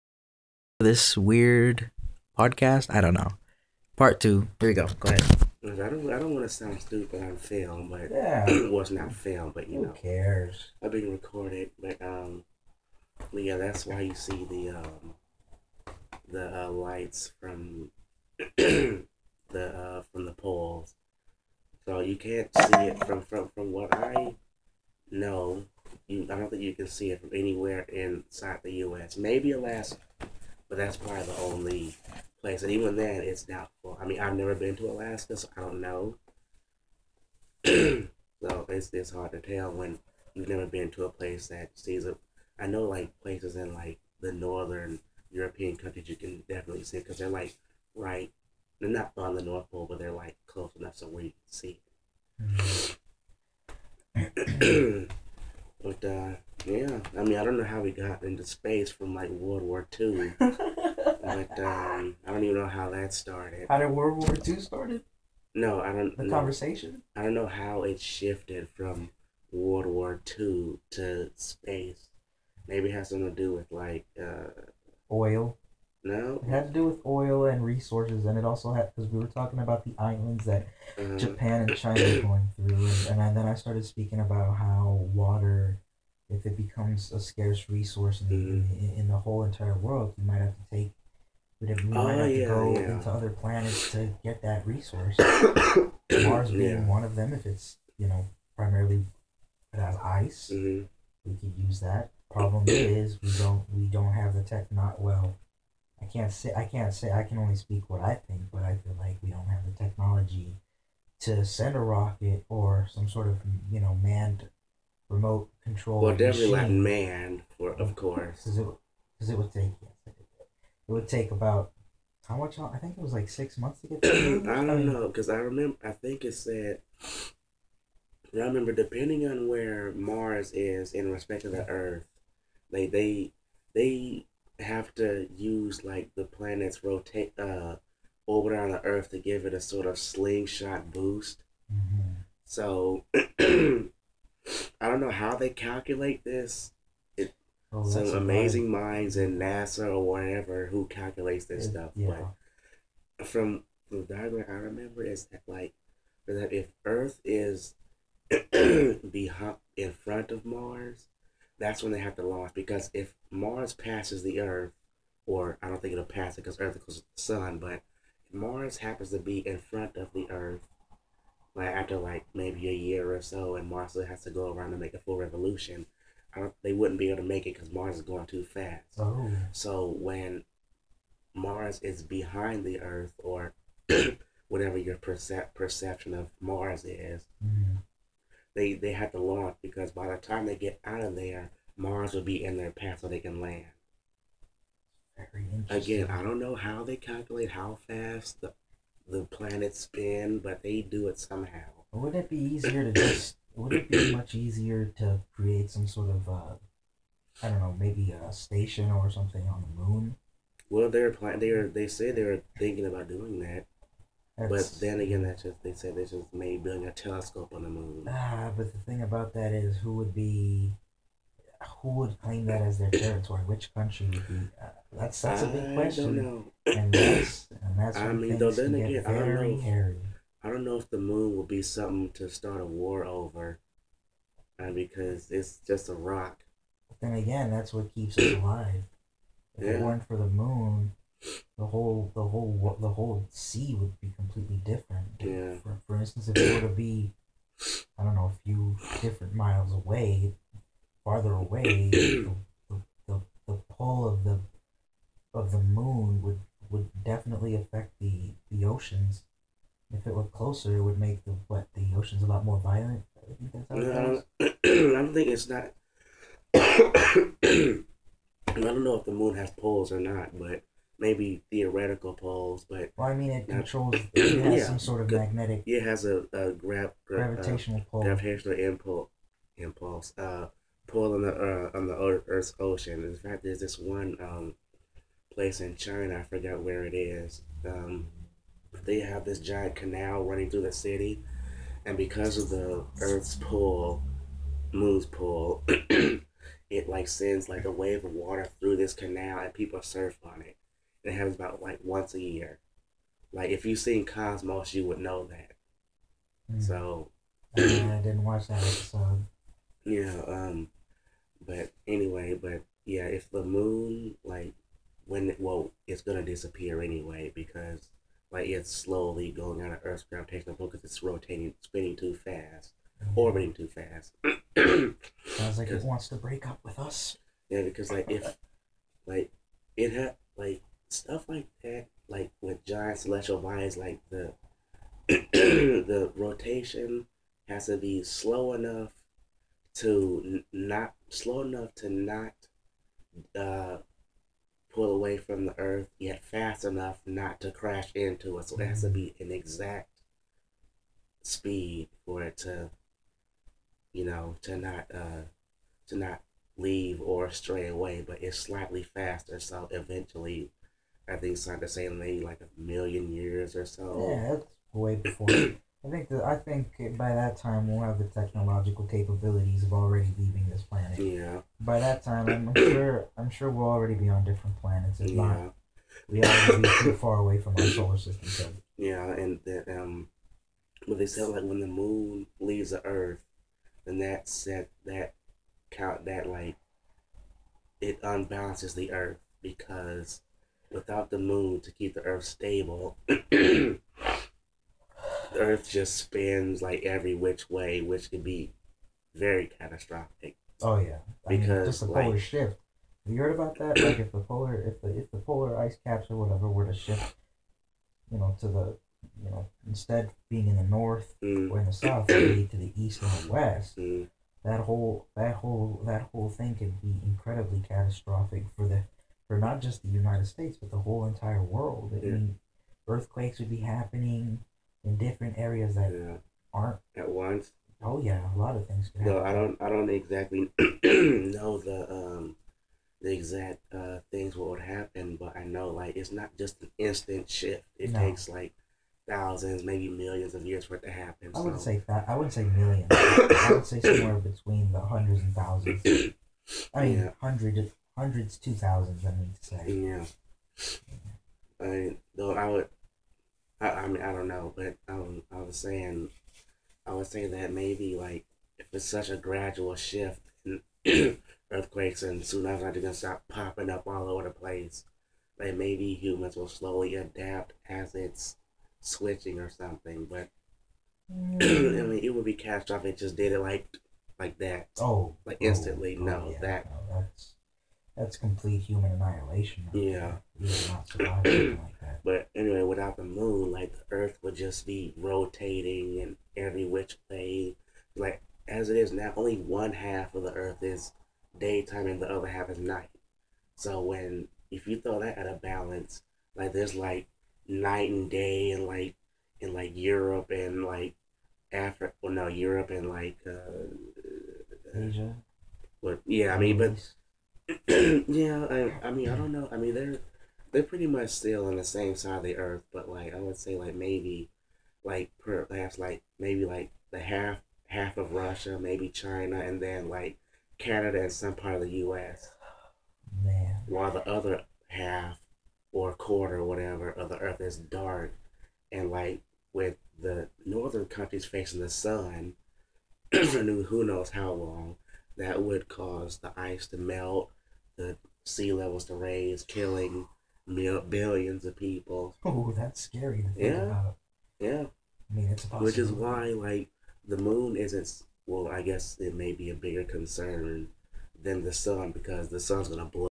Random thoughts 2
Victor Arcadian and Daniel Hirihito
Talking about Astronomy, FDR , WW2 , ETC.